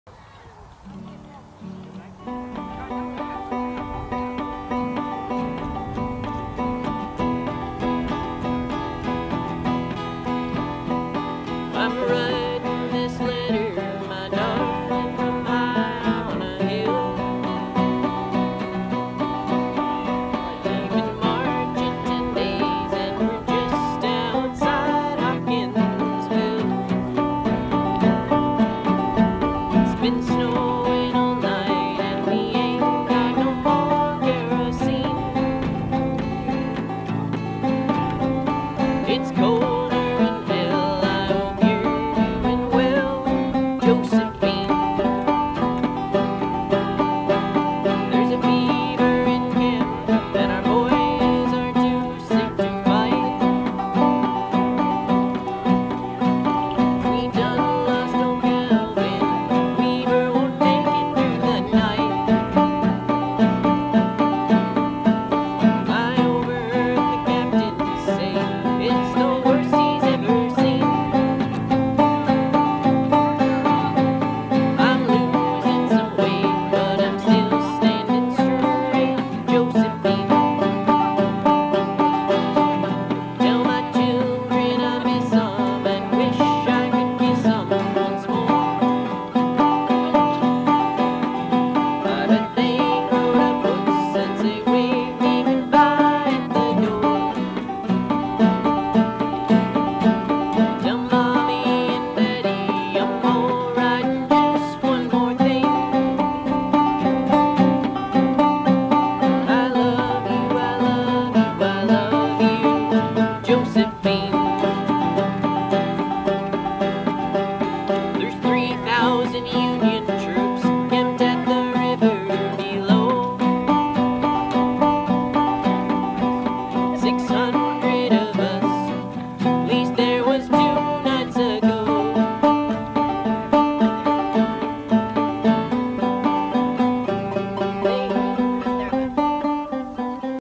NCWC Powerland - Civil War Music
One of the many things I like about photographing and get video of NCWC Reenactments is the Civil War era music.  These two very talented musicians are fantastic,